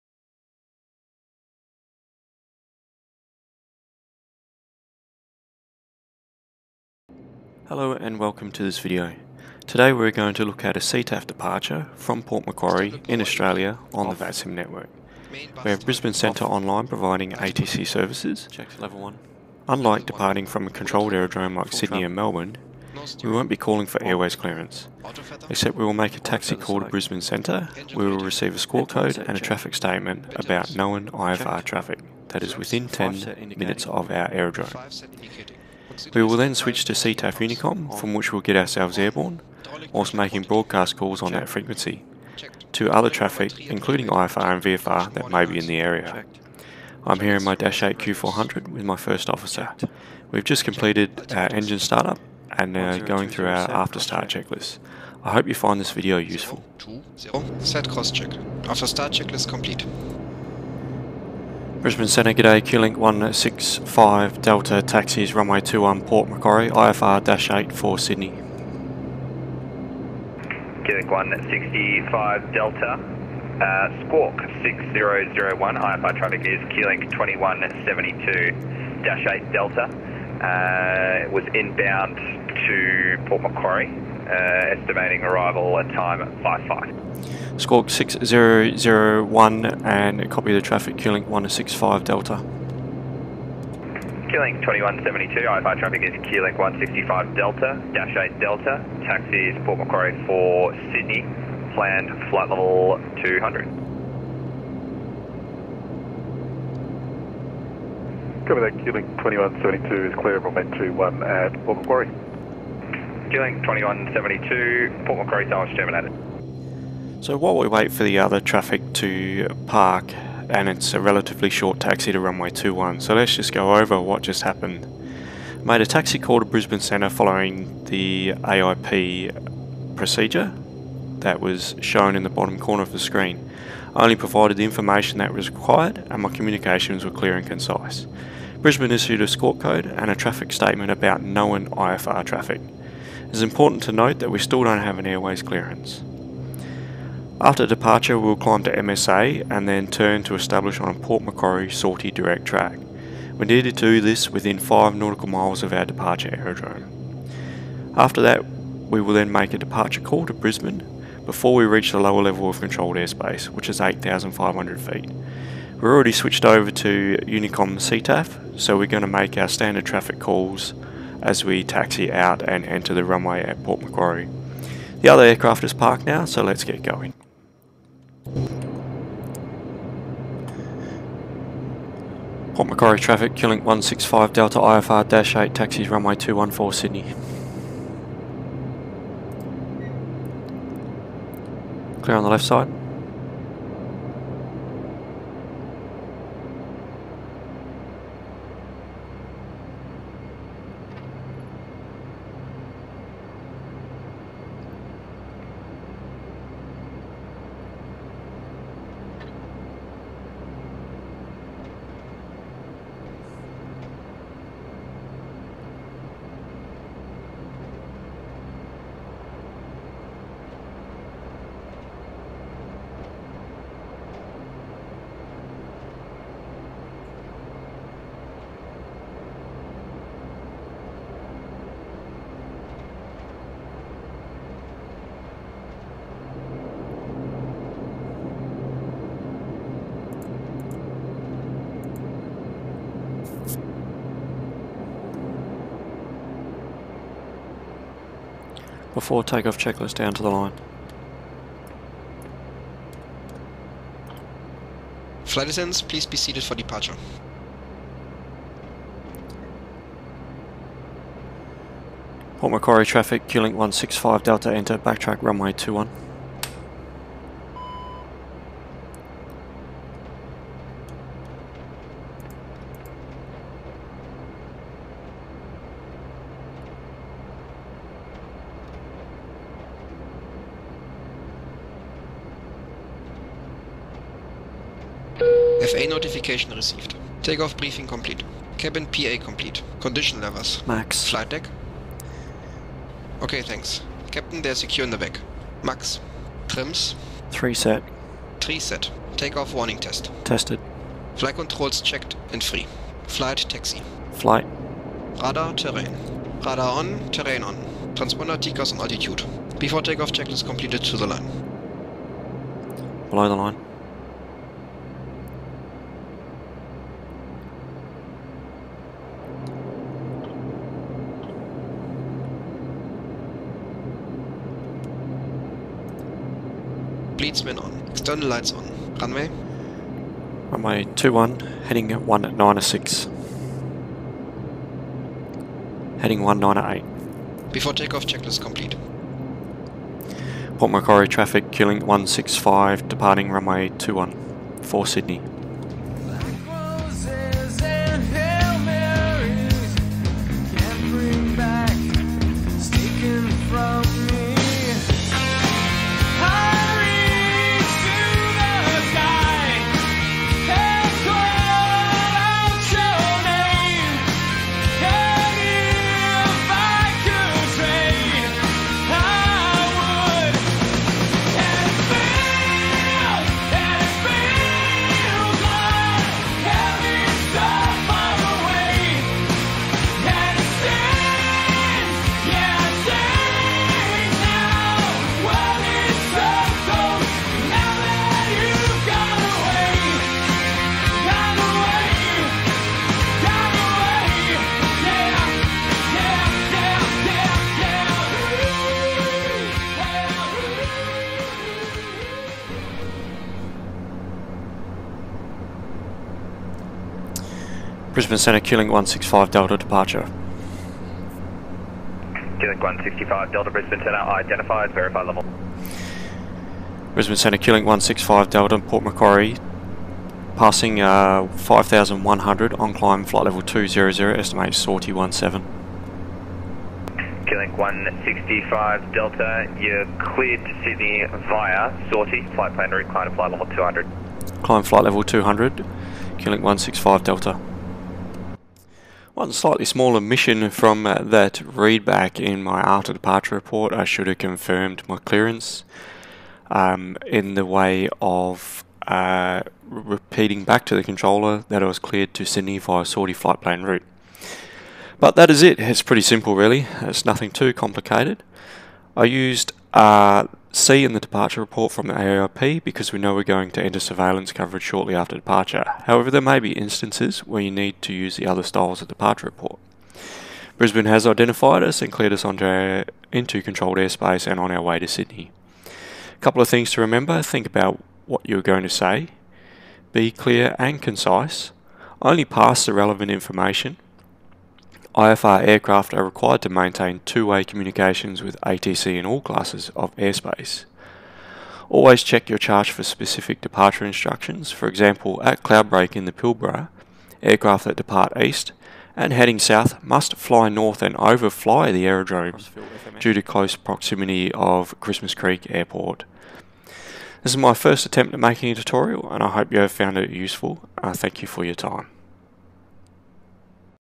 Australian CTAF Departure VATSIM
Departing Port Macquarie in the Majestic Dash 8 Q400, I have taken the opportunity to make a video outlining the communications you make with ATC when departing a non towered aerodrome with overlying Centre controller coverage.